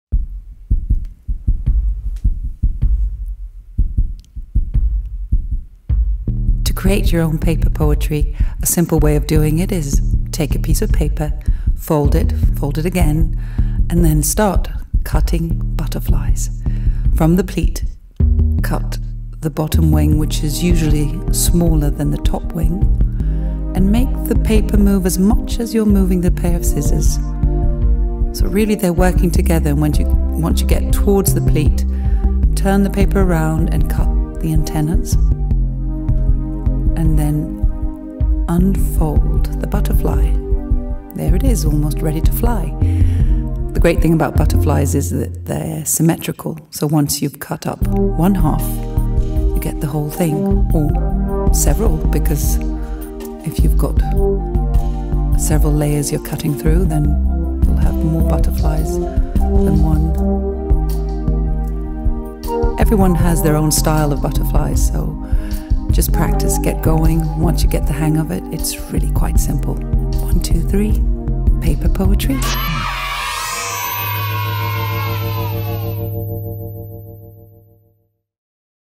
Helene and Simone's cut out butterflies
Twins Helene and Simone Bendix show how to cut out butterflies and create some PAPER POETRY.
The film is made to accompany their book PAPER POETRY.
Cut butterflies in any kind of paper and sprinkle all around you. We have used Betty Soldi's calligraphy and cut it out in paper.
Find your own butterfly style and once you have cut up about 100 almost like practicing your signature, you may be ready to make a butterfly curtain.